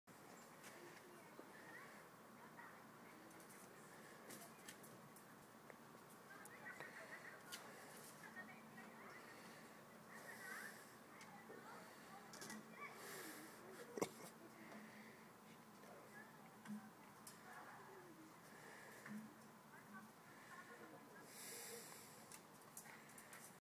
Blind dog trying to get ball off trailer hitch ball
My dog chopper was trying to get the tennis ball off the trailerhitch ball